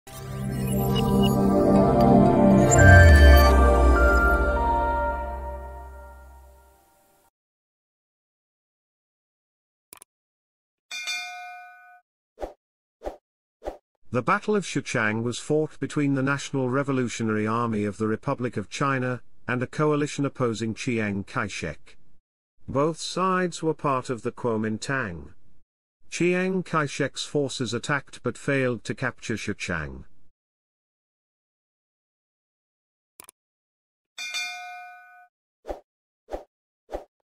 Battle of Xuchang - Wikipedia article
Video summary:
The Battle of Xuchang was fought between the National Revolutionary Army of the Republic of China and a coalition opposing Chiang Kai-shek. Both sides were part of the Kuomintang. Chiang Kai-shek's forces attacked but failed to capture Xuchang.